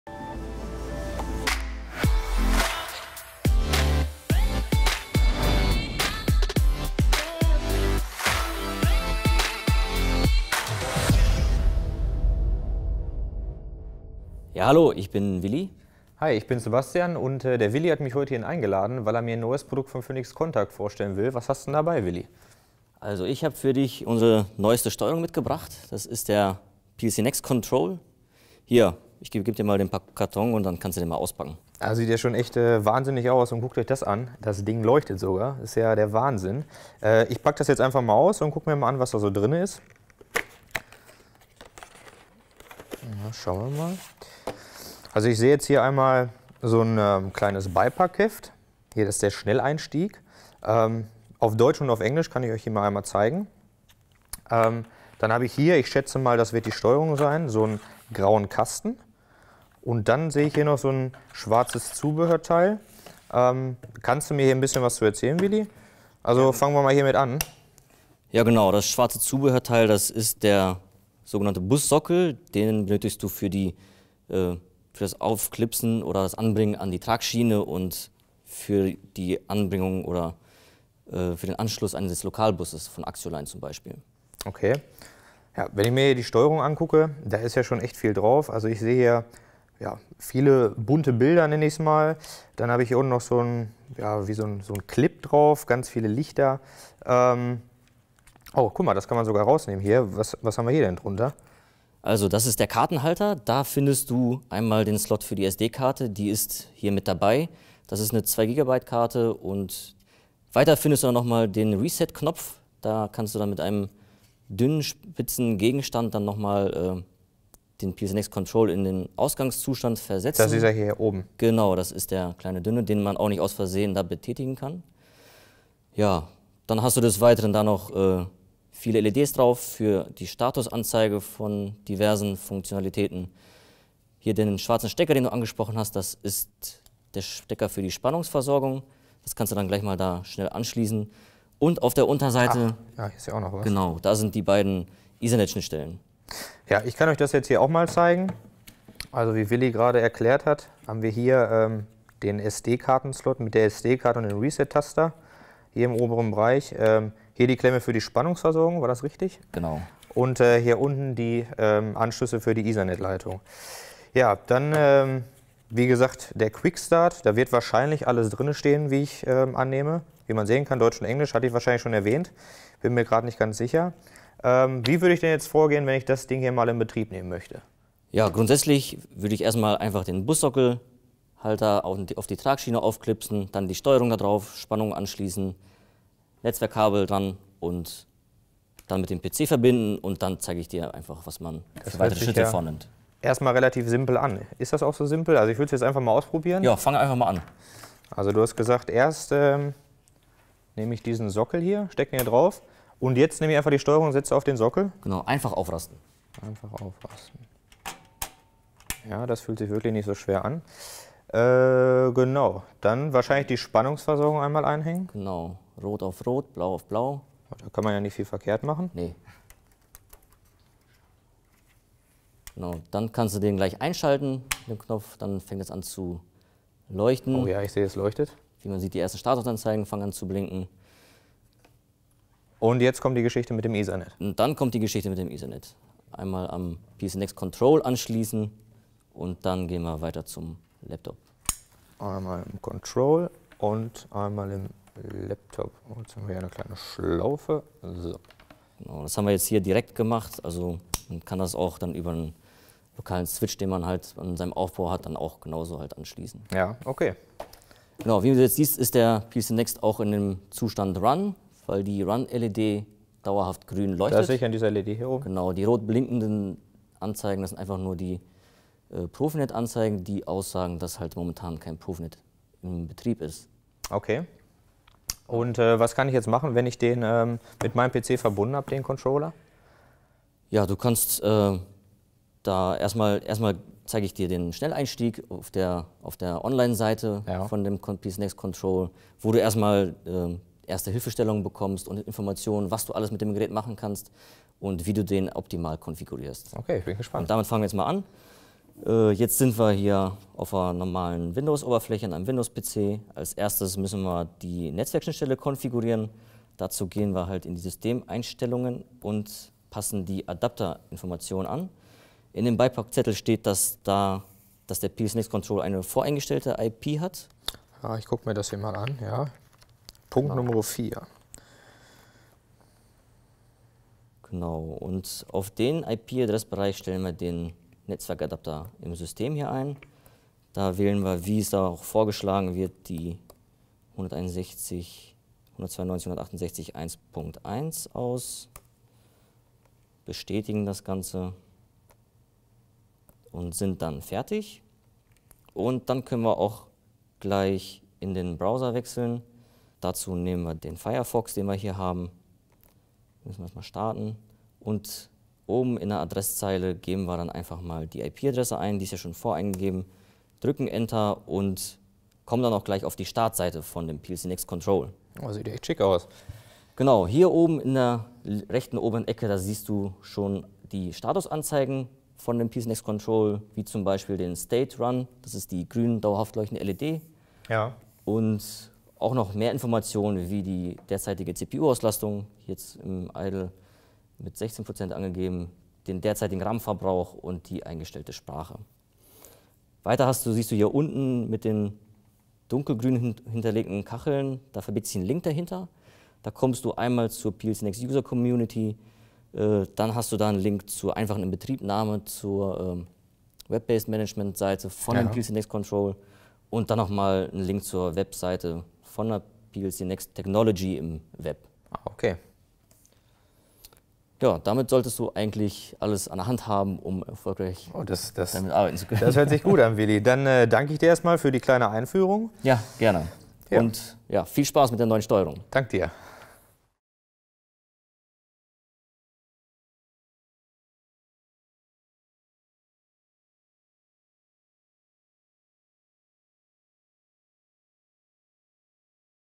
Unboxing der neuen SPS PLCnext Control von PHOENIX CONTACT
▶Unser Unboxing-Video zeigt Ihnen die neue PLCnext Control und ihre einfache Installation.

Die offene speicherprogrammierbare Steuerung PLCnext Technology ermöglicht Ihre Prozessautomatisierung für die Industrie 4.0.